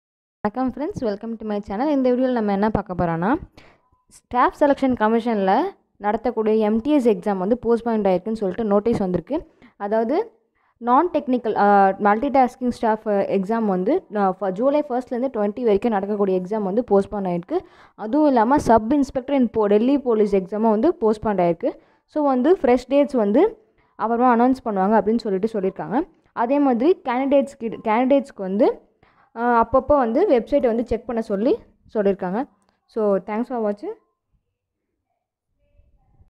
SSC MTS EXAM 2020 POSTPONED in Tamil|New SSC Exam dates|Staff Selection Commission
This video is about ssc mts exam postponed..